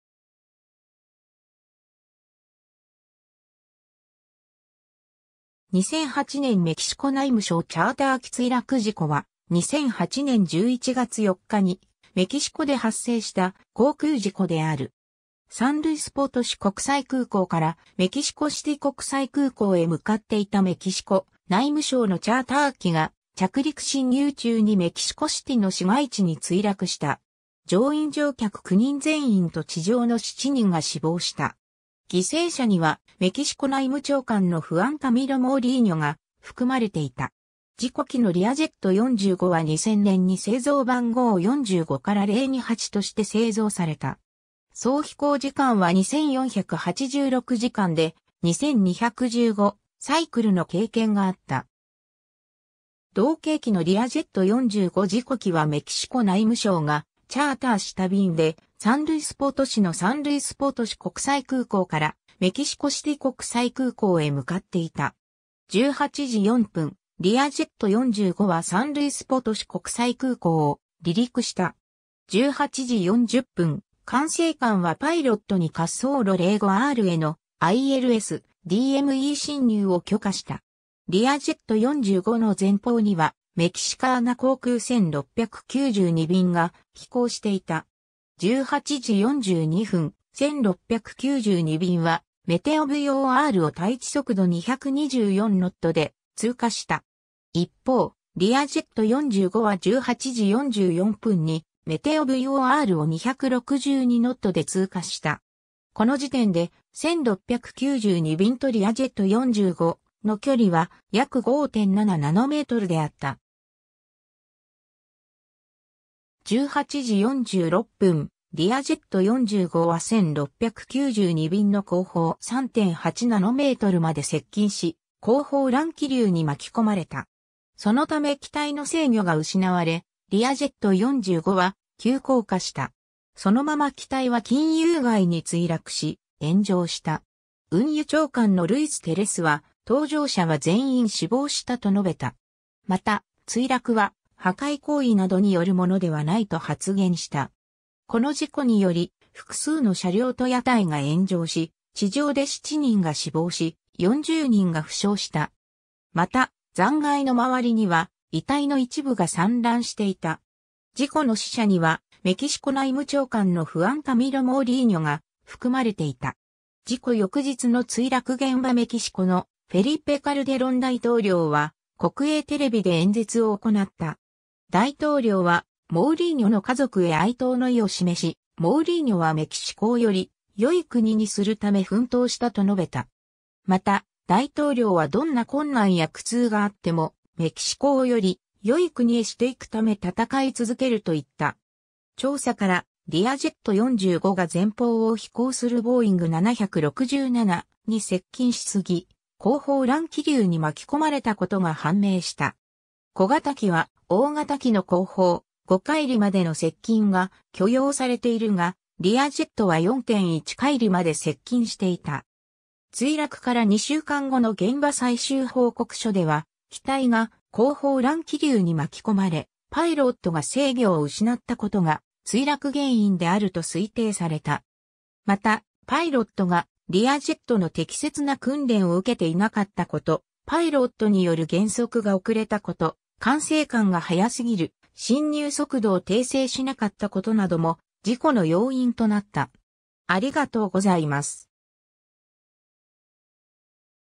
2008年メキシコ内務省チャーター機墜落事故
2008年メキシコ内務省チャーター機墜落事故は、2008年11月4日にメキシコで発生した航空事故である。
サン・ルイス・ポトシ国際空港からメキシコシティ国際空港へ向かっていたメキシコ内務省のチャーター機（リアジェット45）が着陸進入中にメキシコシティの市街地に墜落した。
乗員乗客9人全員と地上の7人が死亡した。
犠牲者にはメキシコ内務長官のフアン・カミロ・モウリーニョが含まれていた。
 事故機のリアジェット45（XC-VMC）は2000年に製造番号45-028として製造された。
総飛行時間は2,486時間で、2,215サイクルの経験があった。
 同型機のリアジェット45 事故機はメキシコ内務省がチャーターした便で、サン・ルイス・ポトシのサン・ルイス・ポトシ国際空港からメキシコシティ国際空港へ向かっていた。
 18時04分、リアジェット45はサン・ルイス・ポトシ国際空港を離陸した。
18時40分、管制官はパイロットに滑走路05RへのILS / DME進入を許可した。
リアジェット45の前方にはメキシカーナ航空1692便（ボーイング767-300）が飛行していた。
18時42分、1692便はメテオVORを対地速度224ノット (415 km/h)で通過した。
一方、リアジェット45は18時44分にメテオVORを262ノット (485 km/h)で通過した。
この時点で、1692便とリアジェット45の距離は約5.7NMであった。
18時46分、リアジェット45は1692便の後方3.8NMまで接近し、後方乱気流に巻き込まれた。
そのため機体の制御が失われ、リアジェット45は急降下した。
そのまま機体は金融街に墜落し、炎上した。
運輸長官のルイス・テレスは搭乗者は全員死亡したと述べた。
また、墜落は破壊行為などによるものではないと発言した。
 この事故により複数の車両と屋台が炎上し、地上で7人が死亡し、40人が負傷した。
また、残骸の周りには遺体の一部が散乱していた。
 事故の死者にはメキシコ内務長官のフアン・カミロ・モウリーニョが含まれていた。
 事故翌日の墜落現場 メキシコのフェリペ・カルデロン大統領は国営テレビで演説を行った。
大統領はモウリーニョの家族へ哀悼の意を示し、モウリーニョはメキシコをより良い国にするため奮闘したと述べた。
また、大統領はどんな困難や苦痛があってもメキシコをより良い国へしていくため戦い続けると言った。
 調査からリアジェット45が前方を飛行するボーイング767に接近しすぎ、後方乱気流に巻き込まれたことが判明した。
小型機は大型機の後方、5海里（9.3km）までの接近が許容されているが、リアジェットは4.1海里（7.6km）まで接近していた。
 墜落から2週間後の現場 最終報告書では機体が後方乱気流に巻き込まれパイロットが制御を失ったことが墜落原因であると推定された。
また、パイロットがリアジェットの適切な訓練を受けていなかったこと、パイロットによる減速が遅れたこと、管制官が早すぎる進入速度を訂正しなかったことなども事故の要因となった。